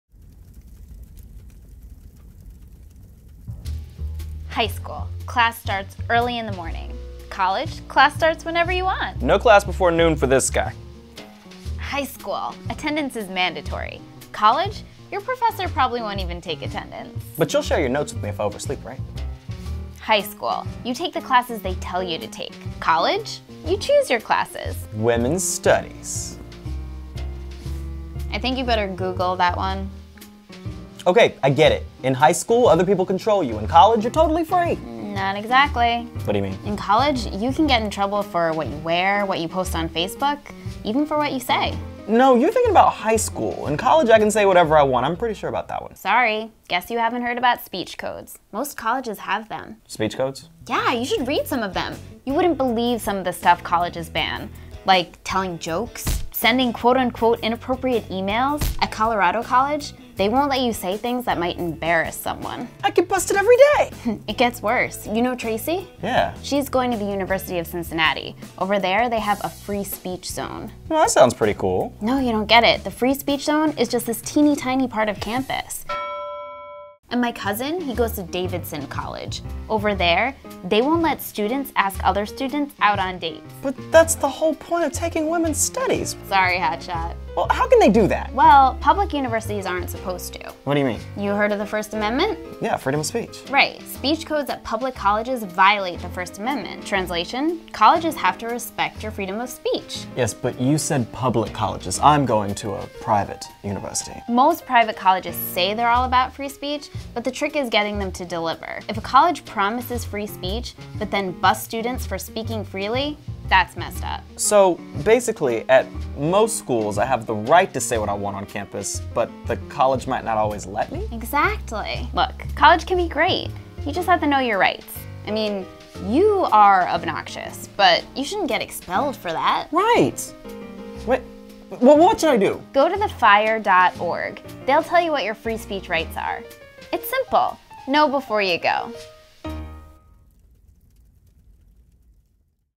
What Every Student Should Know Before Starting College
FIRE's Essay Contest will award $20,000 in scholarships to high school juniors and seniors. to enter. Deadline is January 1, 2014.

Did you know that some colleges and universities restrict student speech rights? Don't be surprised by campus censorship when you get to college — know before you go! to learn more. For more information on the cases and codes included in the video,

Produced by Ted Balaker. Music by Audionautix.com.